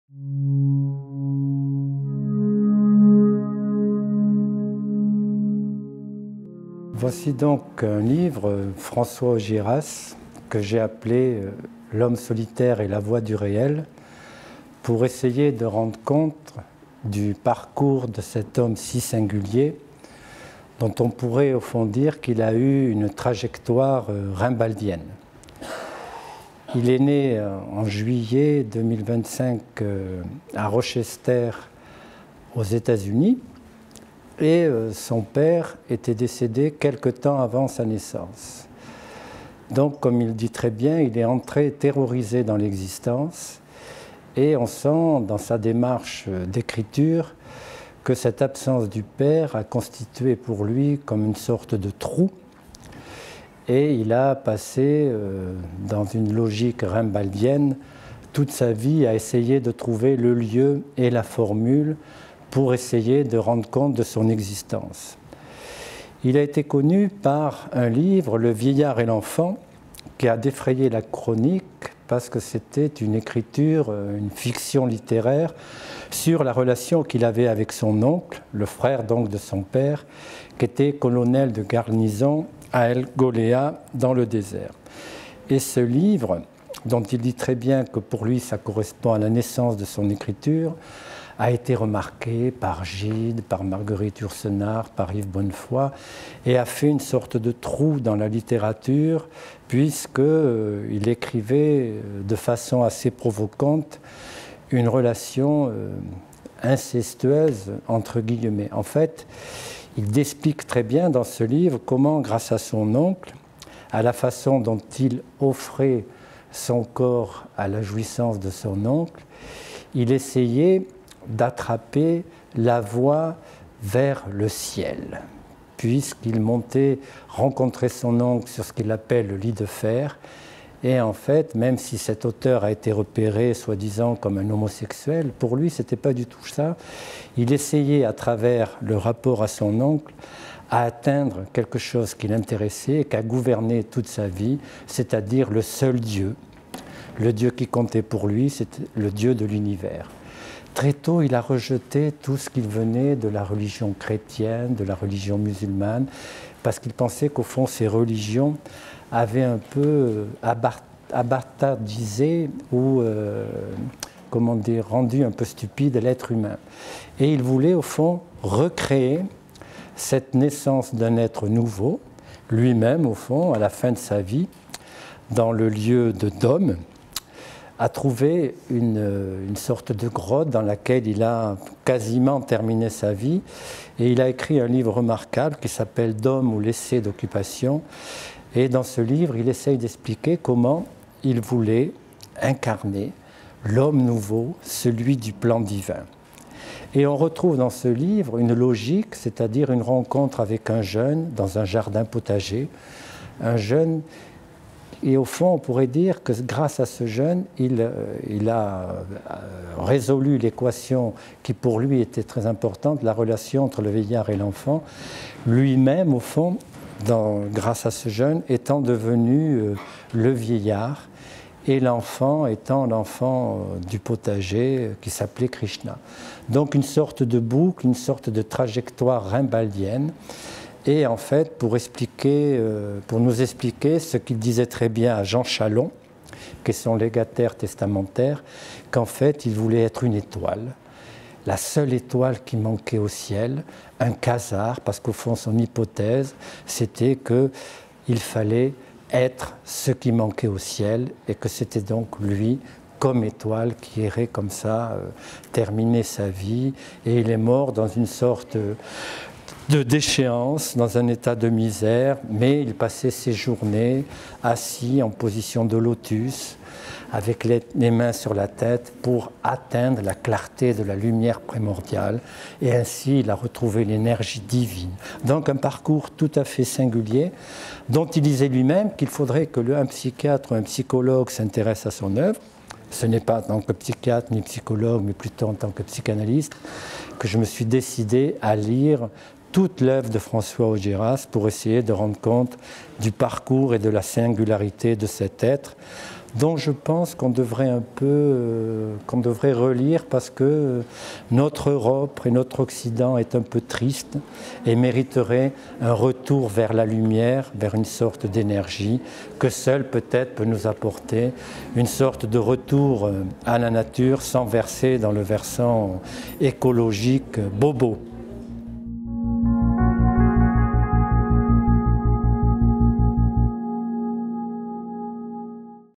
Philippe Lacadée - François Augiéras : l'homme solitaire et la voie du réel
Philippe Lacadée vous présente son ouvrage "François Augiéras : l'homme solitaire et la voie du réel" aux éditions Michèle.